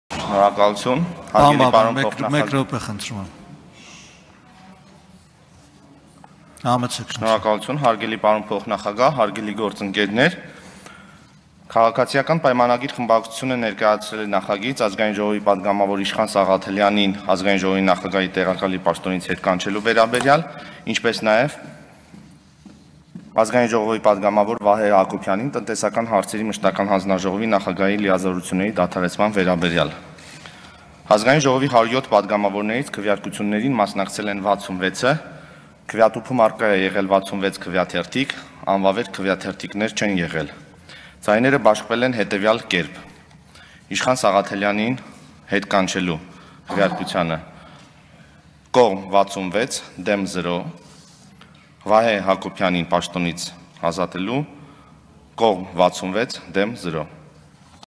Իշխան Սաղաթելյանն ու Վահե Հակոբյանը պաշտոնազրկվեցին՝ 66 կողմ, 0 դեմ ձայներով
Իշխան Սաղաթելյանին և Վահե Հակոբյանին պաշտոններից հետ կանչելու որոշման նախագծին կողմ է քվեարկել 66 պատգամավոր, դեմ՝ 0